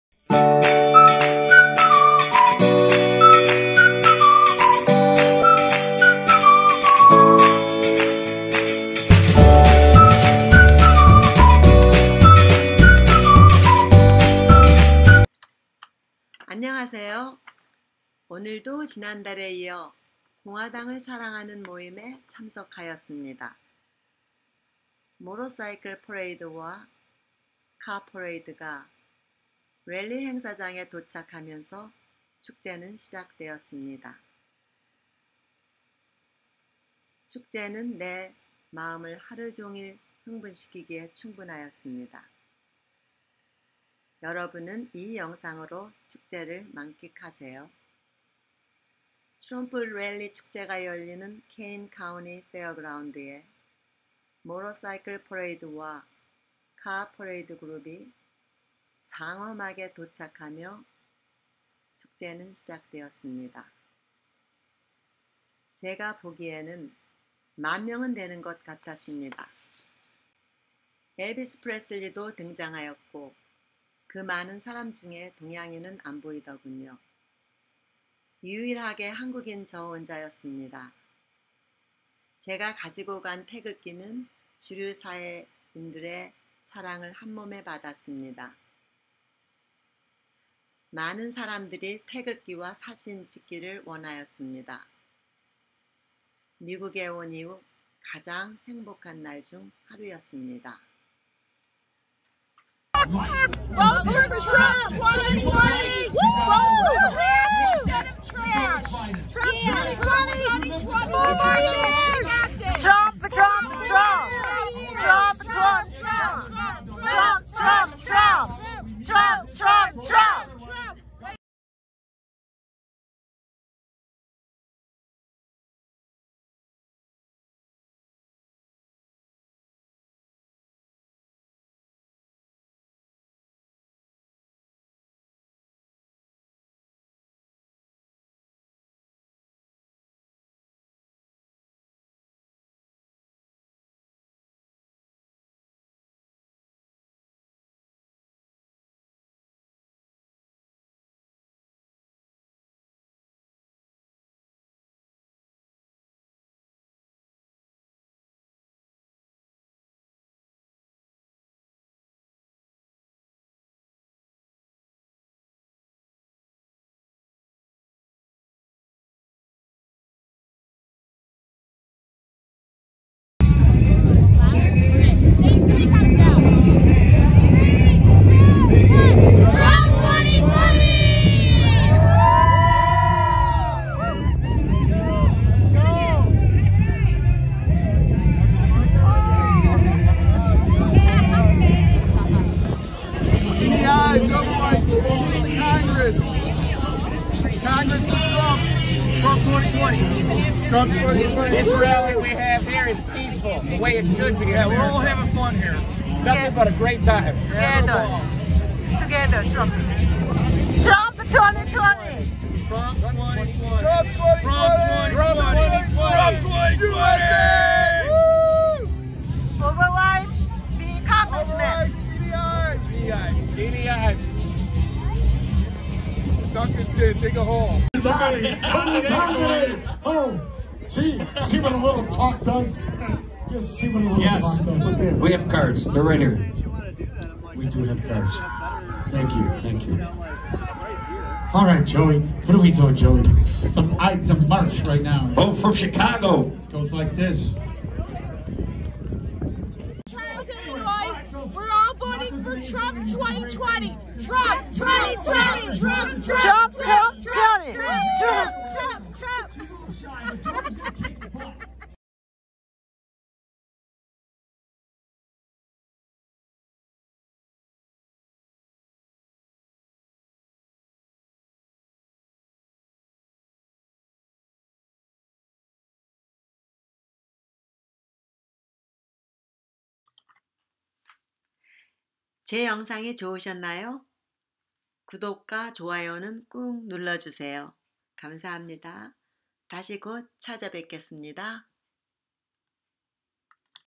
Unbelievable Trump Rally at Kane County Fairground Chicago (9-27-2020)
Unbelievable Trump Rally Kane County Fairground, IL.
There were about 1,0000 people.